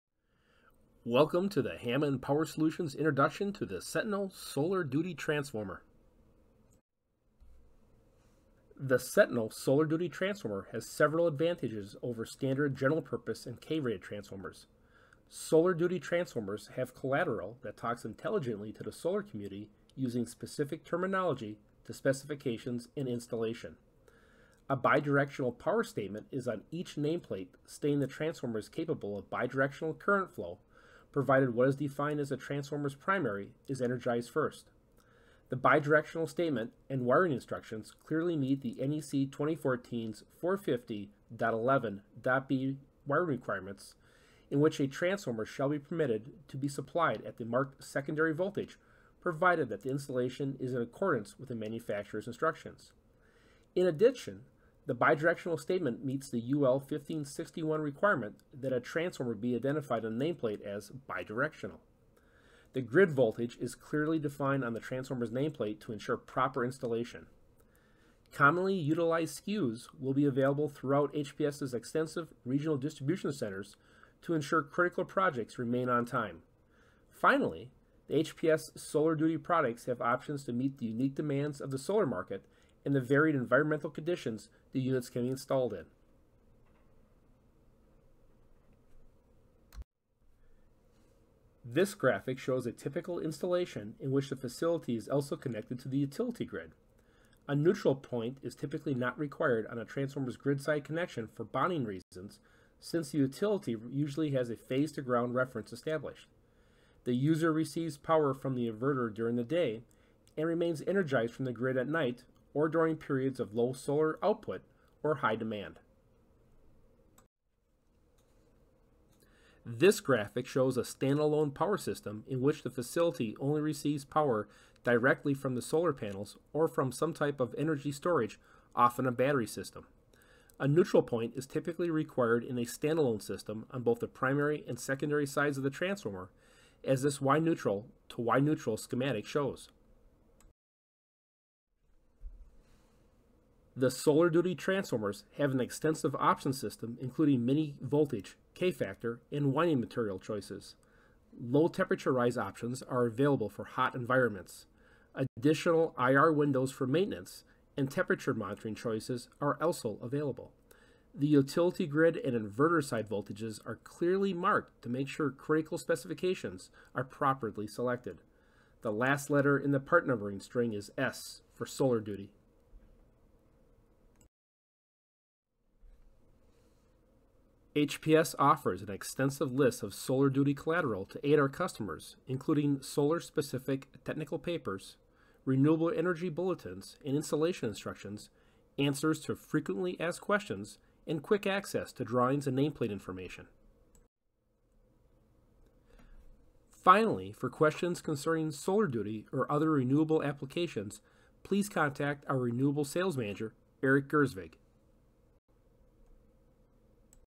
HPS Sentinel™ Solar Duty Distribution Transformer
HPS offers Sentinel Solar Duty transformers are designed for the renewable energy industry. These transformers are designed for applications where voltage adjustments are necessary between the solar generation system and the utility service. Isolation transformers are traditionally used where
power flows in a single direction to feed a load.